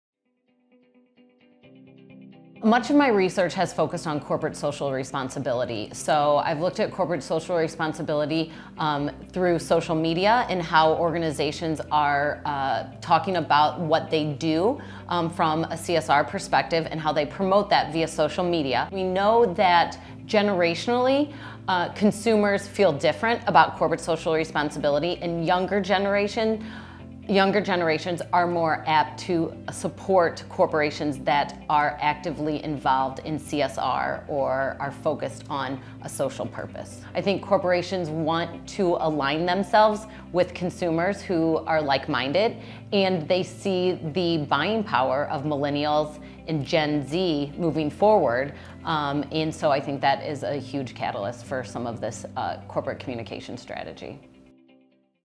Kati Berg
Dr. Kati Berg discusses the importance of addressing social corporate responsibility in the field of strategic communication.